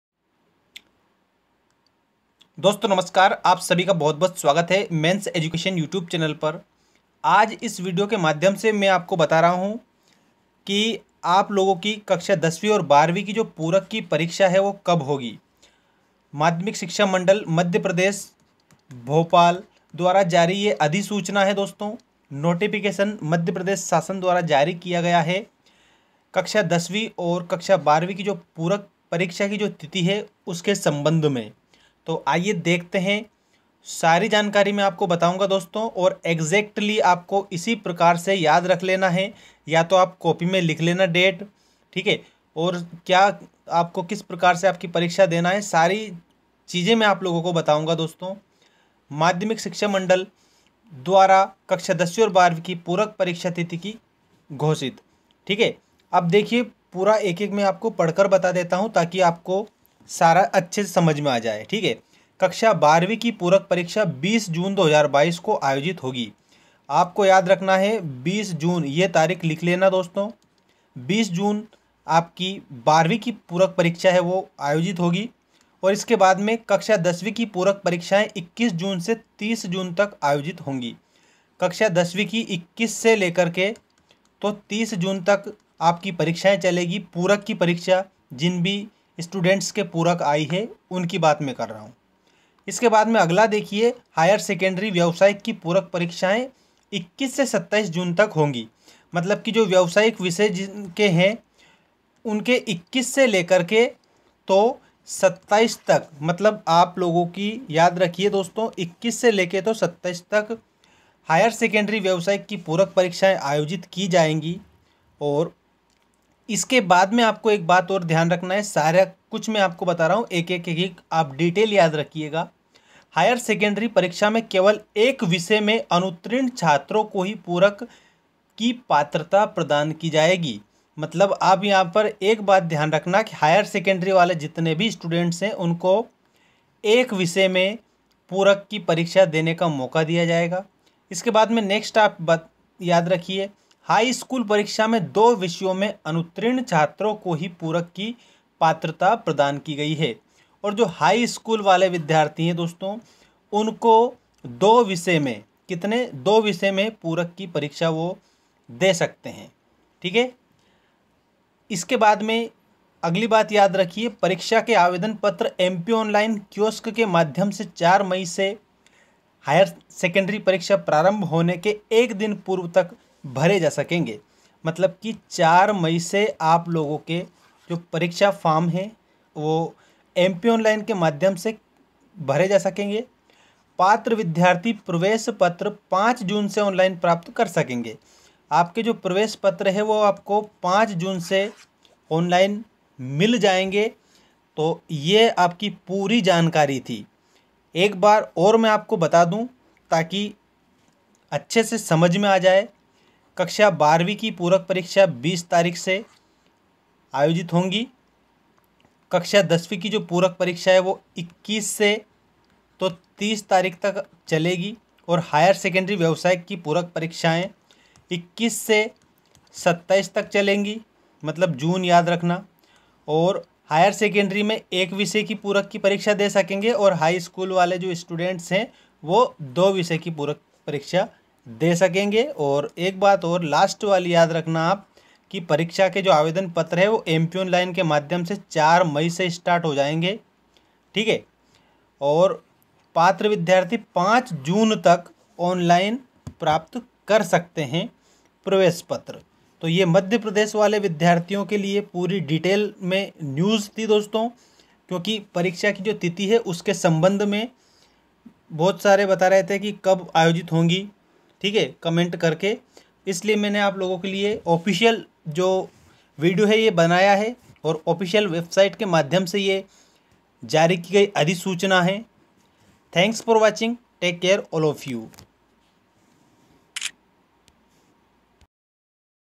एमपी 10वीं व 12वीं पूरक परीक्षा 2022 की तिथि हुई घोषित। Suplymentry exam Date class 10th,12th mp
class 10th mp board 2022 supplementary exam date notification
class 12th mpboard supplementary exam date 2022
mpbse class 10th 12th supplementary exam date 2022